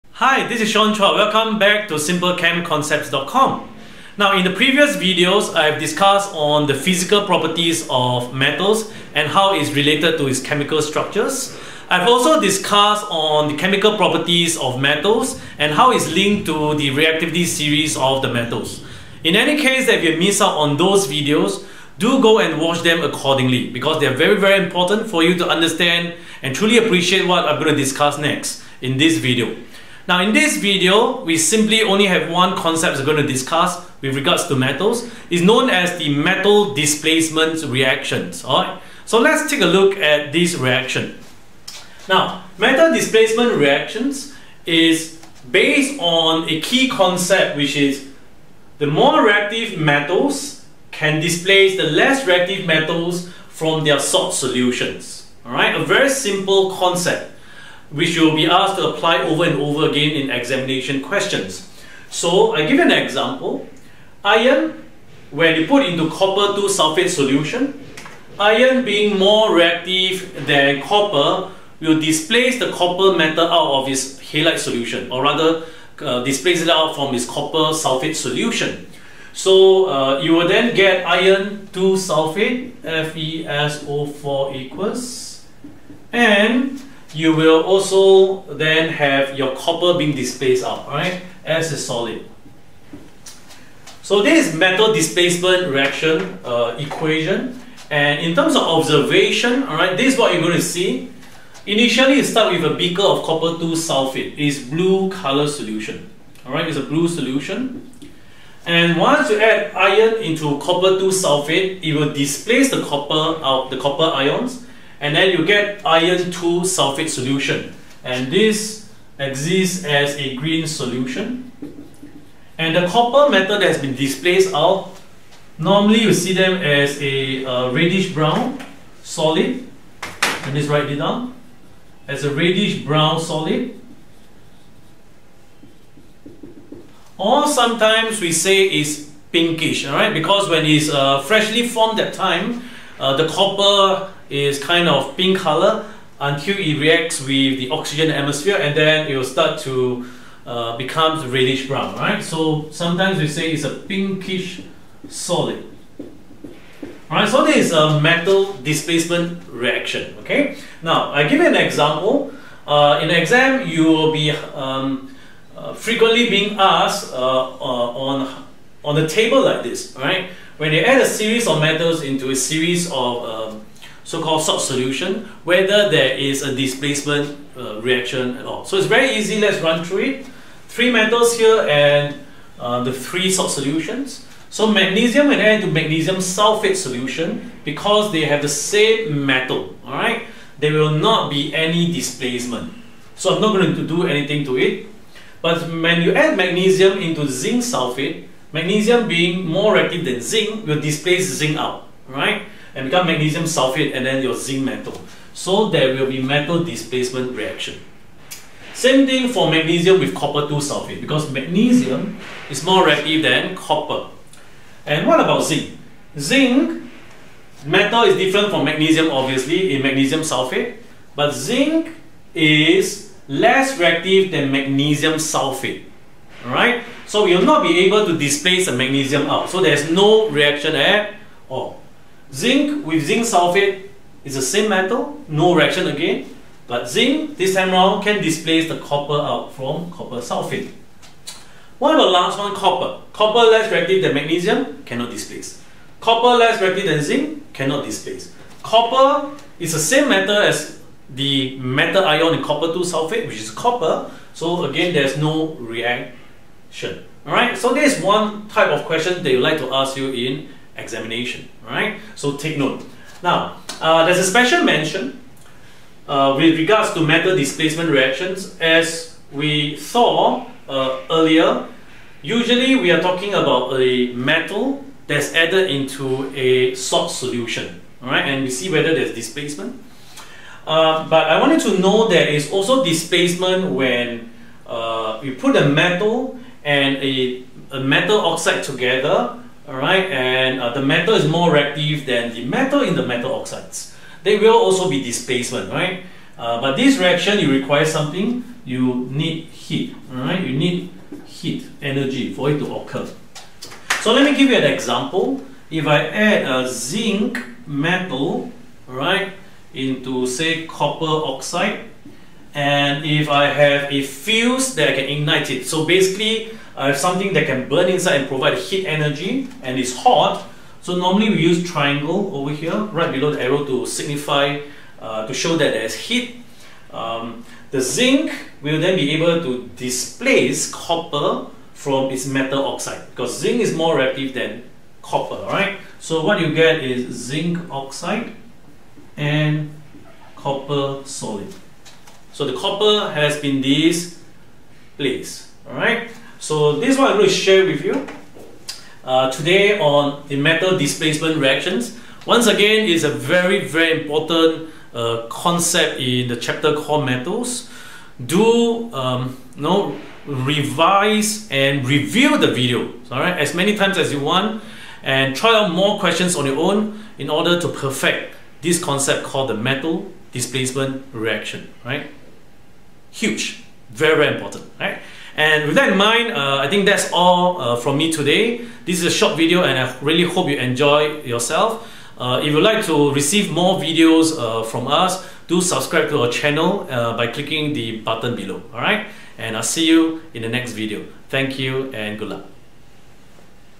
O Level Pure Chemistry. IP Chemistry: Metal Displacement Reactions
In this video, you will learn:

#1: Key concept: The more reactive metals can displace the less reactive metals from their salt solutions.
#2: Displacement of metal from their salt solutions
#3: Reaction between metal & metal oxide in the presence of heat or fuse

I hope you find the content easy for your understanding and that this video will be part of your Chemistry Notes to master the topic on Metals.

For more O Level Pure Chemistry.